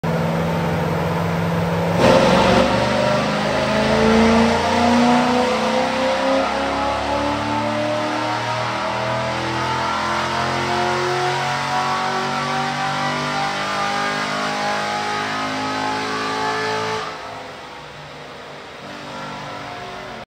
2019 Chevrolet Corvette Grandsport - A8 - Custom Dyno Tune
2019 Chevrolet Corvette Grandsport - A8

Jake Airbox - KOOKS w/ HF Cats - NPP

Tuned by Brett @ Pray Perfomance

419 RWHP & 419 RWTQ SAE